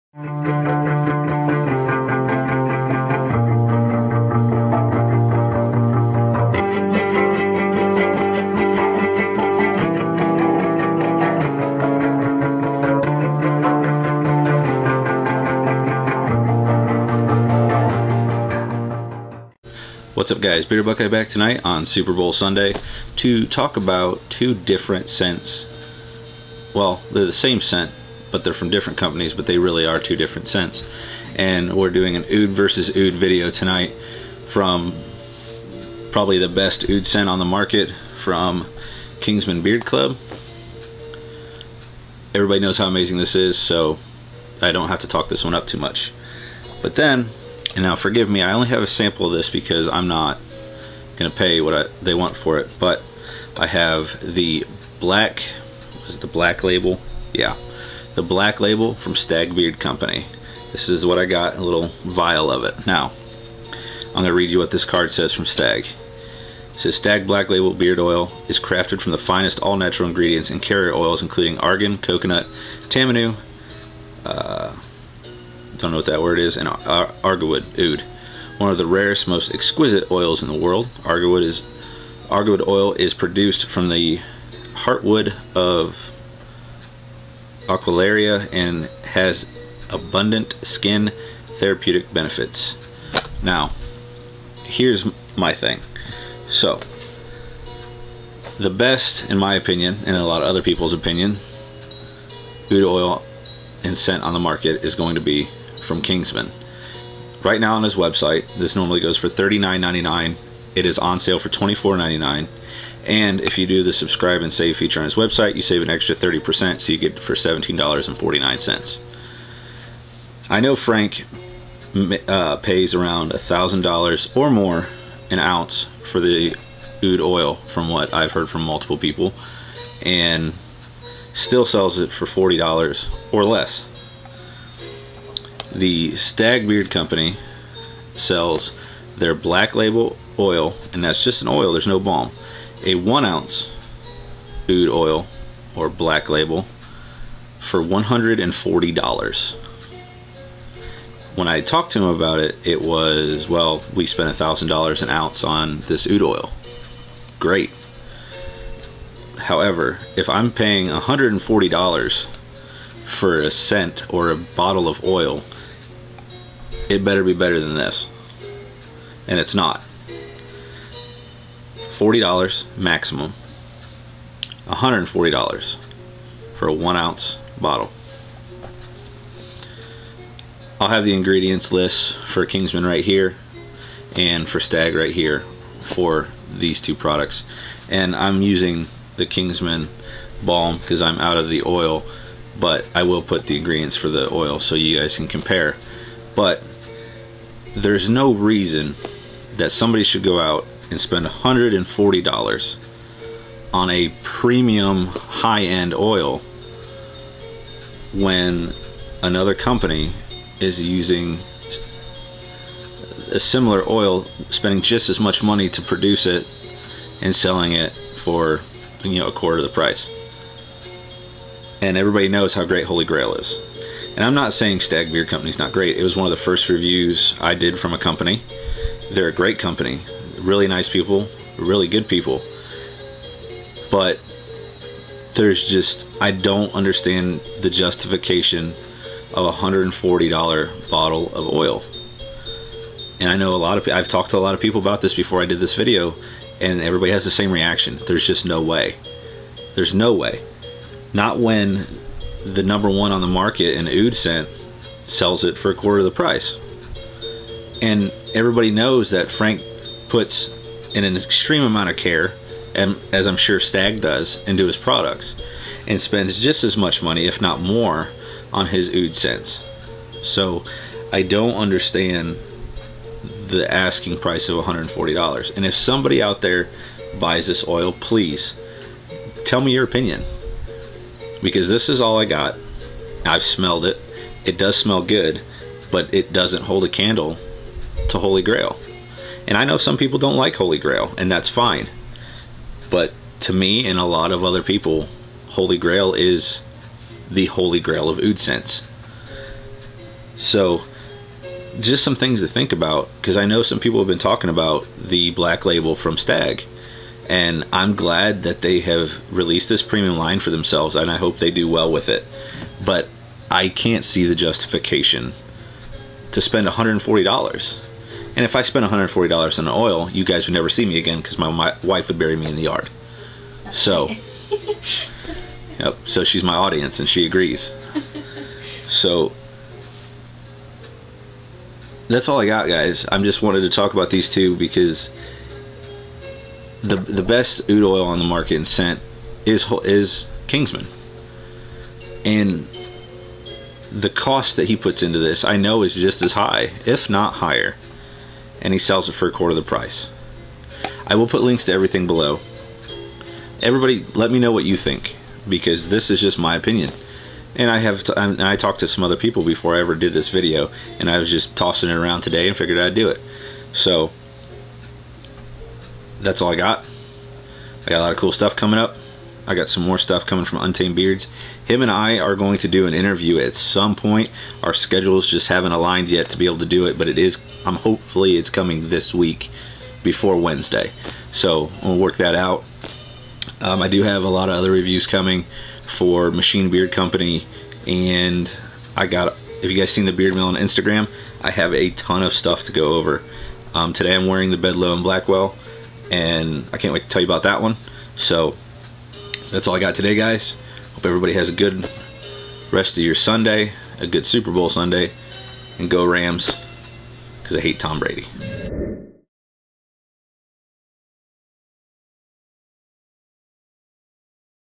Kingsmen Beard Club vs Stag Beard | Oud vs Oud
Does a higher price mean you’re getting a better product? I talked about that with two different Oud scents from two great companies.